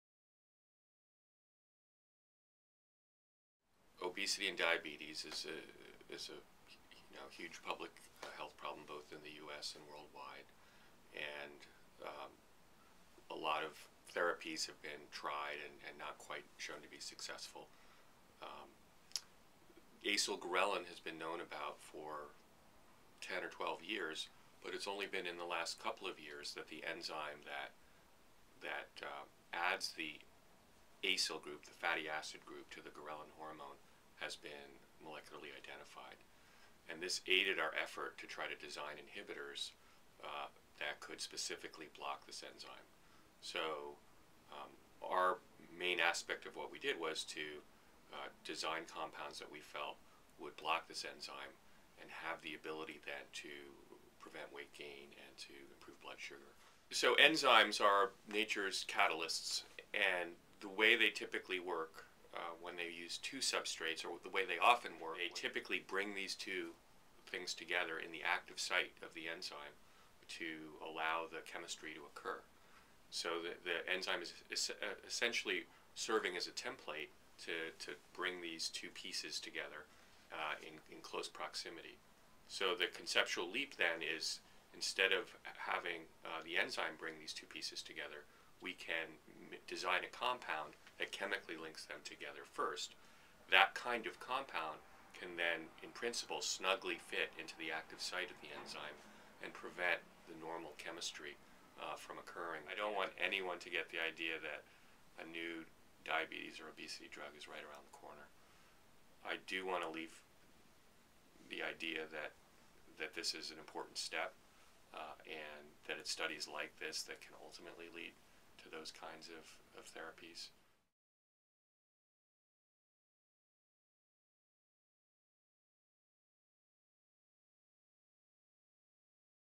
Phil Cole | Developing a Drug That May Potentially Treat Obesity and Diabetes
Phil Cole